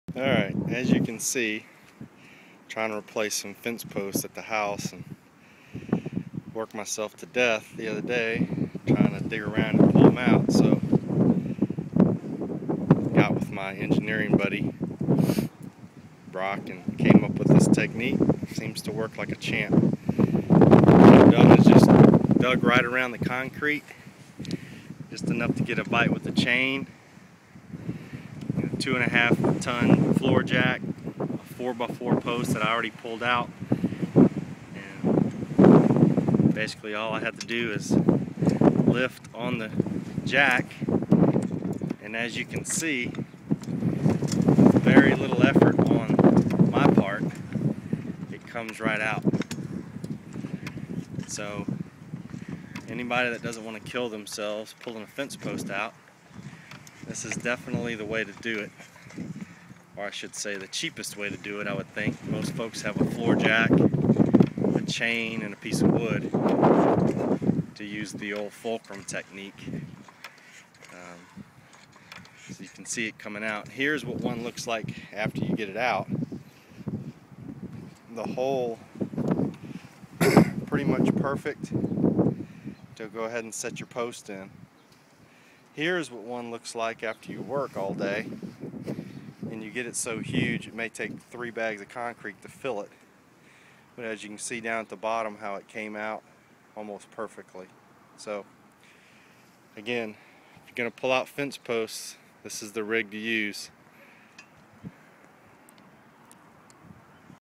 Easy way to remove fence post
This is by far the cheapest and easiest way to remove fence post without the high cost of tools/materials.